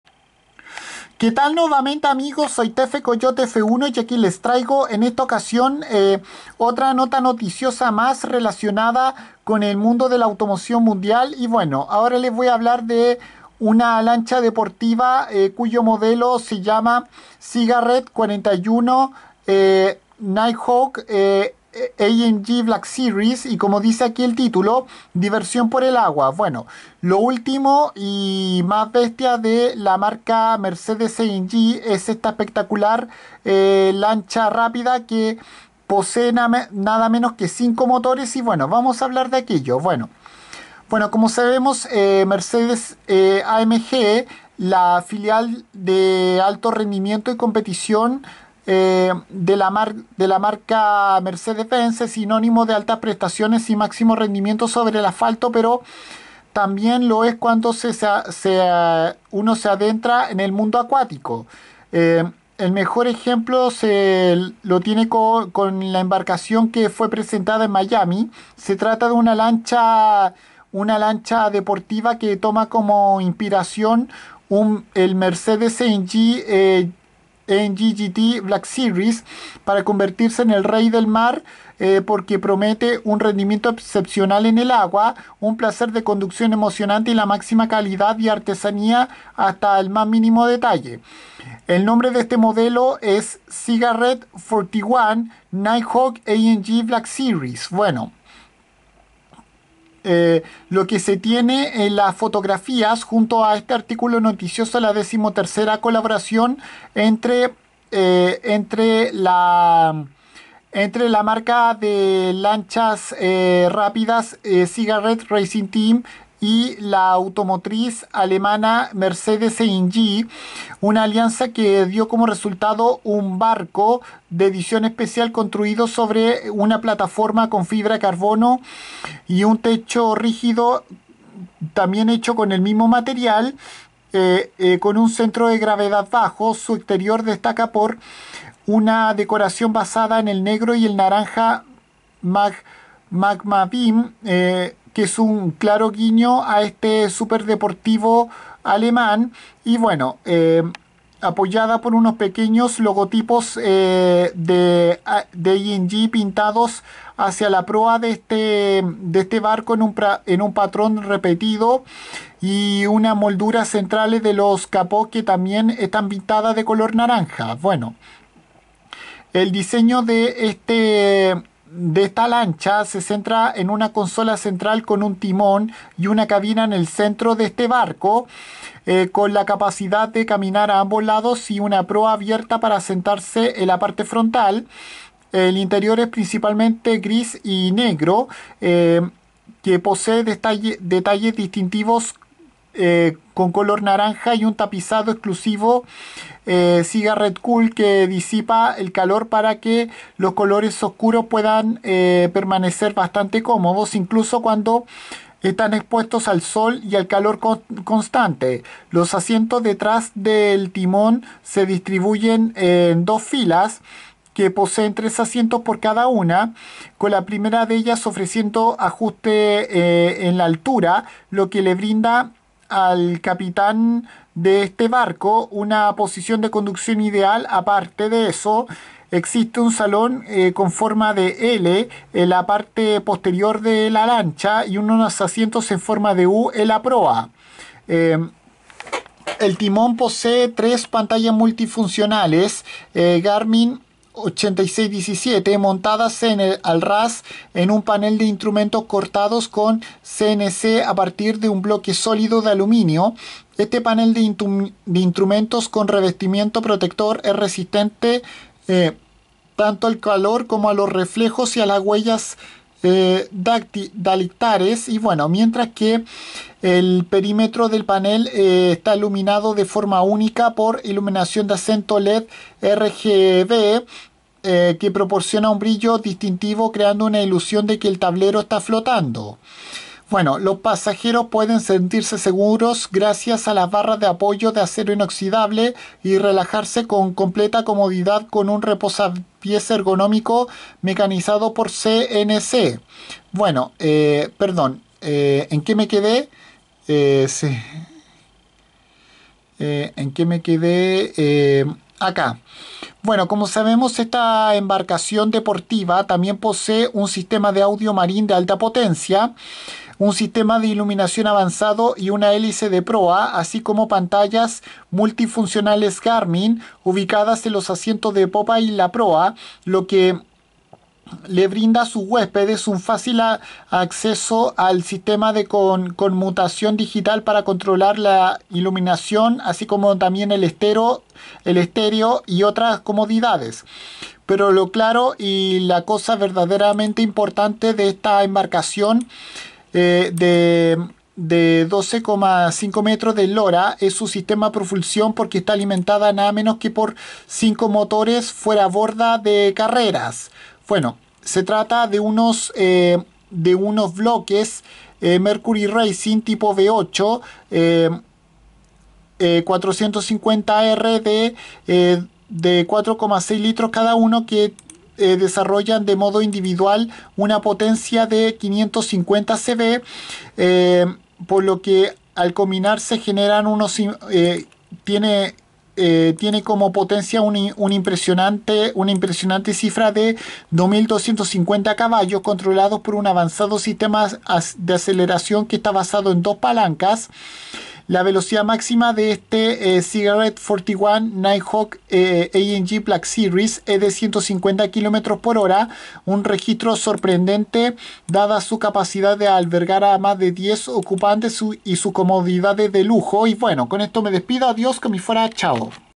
Cigarrette 41' Nighthawk AMG Black Series información
En este vídeo les menciono esta lancha de velocidad inspirada en el diseño de la máquina deportiva alemana de la estrella